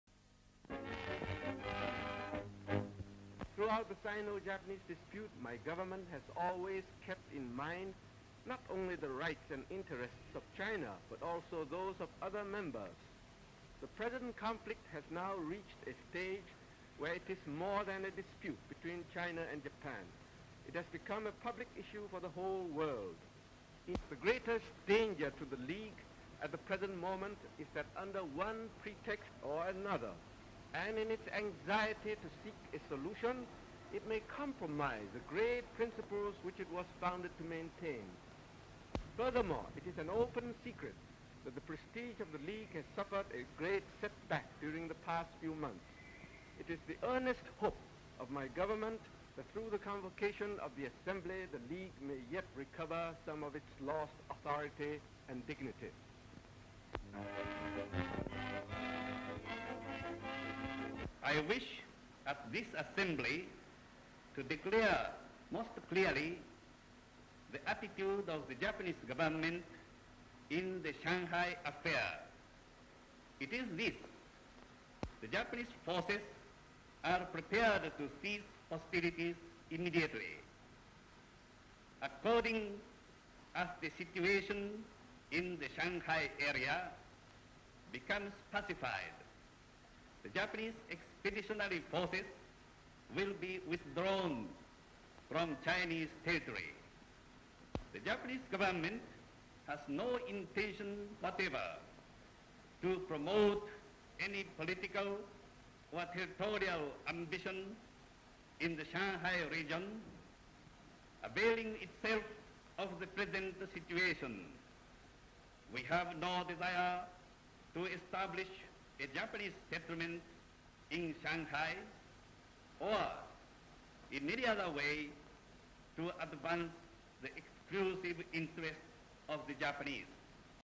The Chinese and Japanese delegates to the League of Nations state tjheir positions on the ...(1934)
The Chinese and Japanese delegates to the League of Nations state their positions in the Sino-Japanese dispute.

(shotlist created 2010)

Full Description:

SLATE INFORMATION - THIS FAR EAST AFFAIR

Geneva, Switzerland

Chinese League of Nations delegate Dr. Yen speaking (English)

" Throughout the Sino-Japanese dispute, my government has always kept in mind , not only the rights and interests of China but also those of other members .The present conflict has now reached a stage where it is more than a dispute between China and Japan , it has become  a public issue for the whole world .The greatest danger to the League at the present moment , is that under one pretext or another and in it's anxiety to  seek a solution it may compromise the great principles which it was founded to maintain .Furthermore it is an open secret that the prestige of the League has suffered a great setback during the past few months. It is the earnest hope of my government through the congregation of the assembly , that the League may yet recover some of its lost parity and dignity."

SLATE - MR MATSUDAIRA PUT'S JAPANESE VIEWPOINT

Japanese delegate Mr Matsudaira saying

" I wish at this assembly , to declare most clearly the attiitude of the Japanese government in the Shanghai affair. It is this - the Japanese forces are prepared to cease hostilities immediately .According to the situation in the Shanghai area,  the Japanese expeditionary forces will be withdrawn from Chinese territory .. The Japanese commandments have no intention whatever to promote any political or territorial ambitions in the Shanghai region, availing itself of the present situation .We have no desire to establish a Japanese settlement in Shanghai or in any other way to advance the exclusive interests of the Japanese. "
Background: The Chinese and Japanese delegates to the League of Nations state their positions in the Sino-Japanese dispute.

(shotlist created 2010)